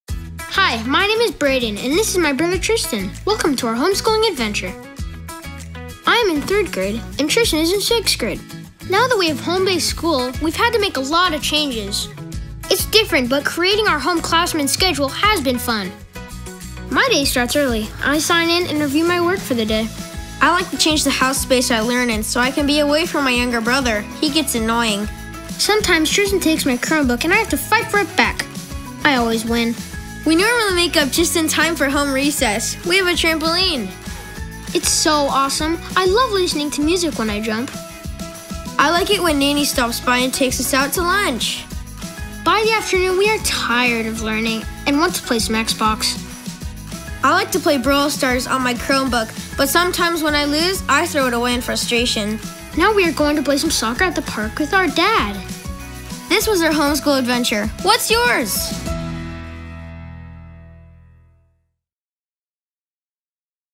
Homeschooling Adventures with Gumdrop
Take-home device initiatives are requiring students and teachers to learn and connect from home, so Gumdrop decided to capture a day in the life of some students and their laptops during homeschooling. Our research shows that take-home device programs see a drastic increase in device damage. Gumdrop rugged cases are here to help! Our laptop and tablet cases are designed specifically for K-12 so they can ensure the safety of your student and teacher devices, even with take-home programs.